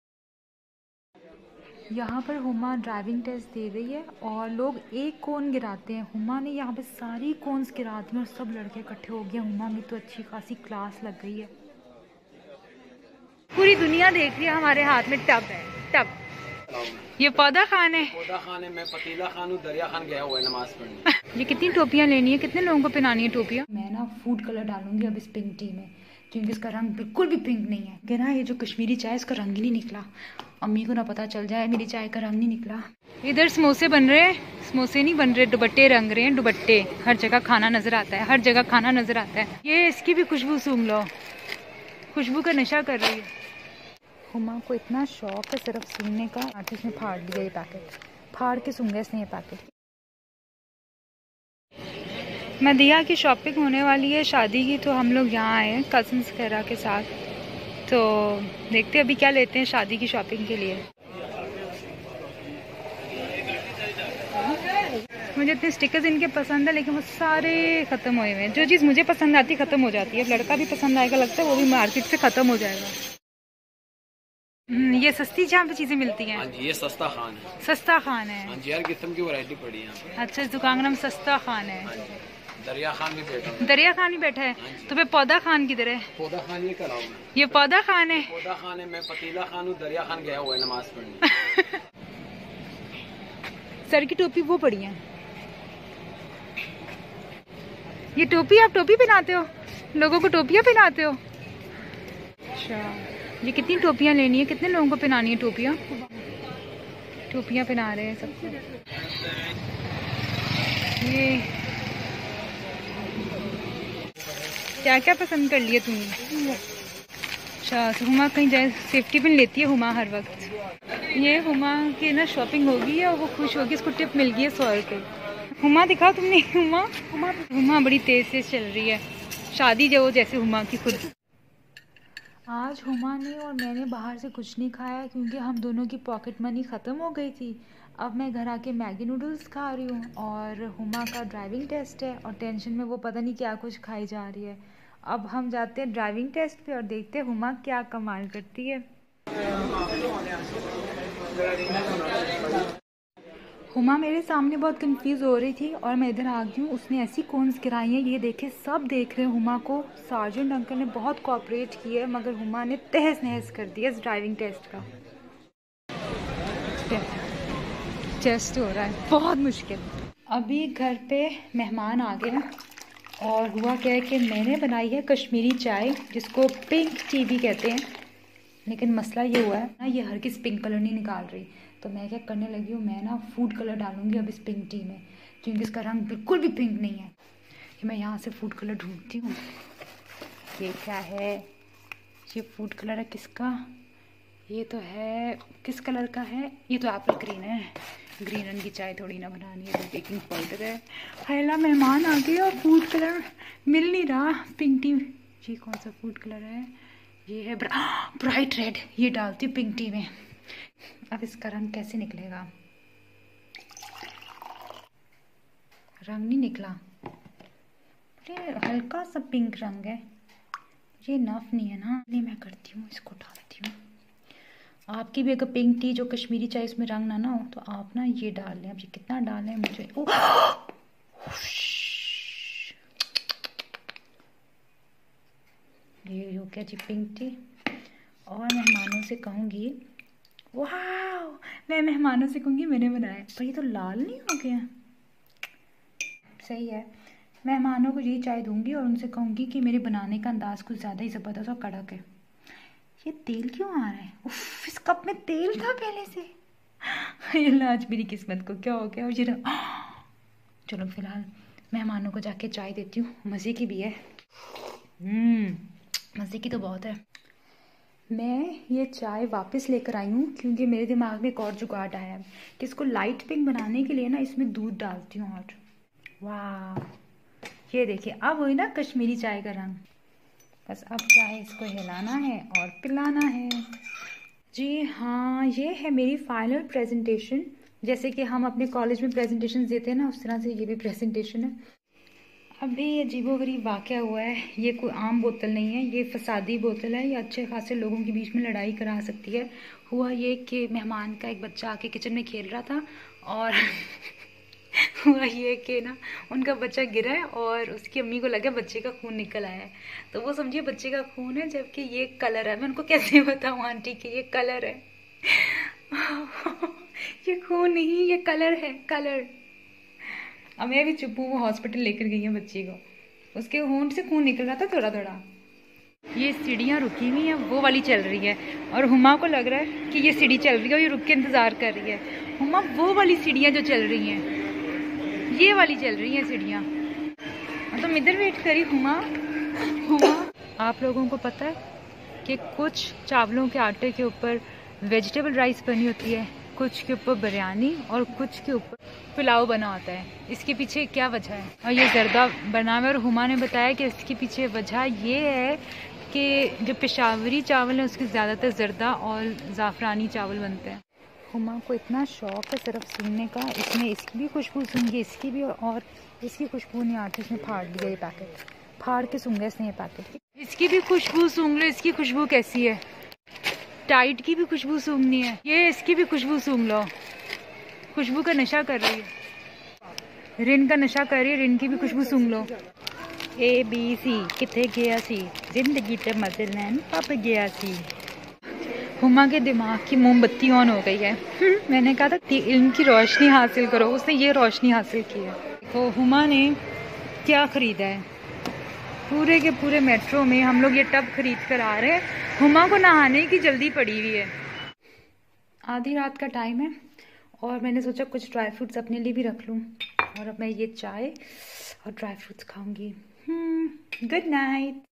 Driving Test Failed 😬 Dawat per Ghalti 🥴 Shadi Shopping 🛍️
Driving Test mein Fail 🥴 Driving License nahi milna ab 😲 Mehman Ki Dawat per ghalti Kiya hui? 🥺 Shadi ki Shopping 🛍️ with yummy food 😋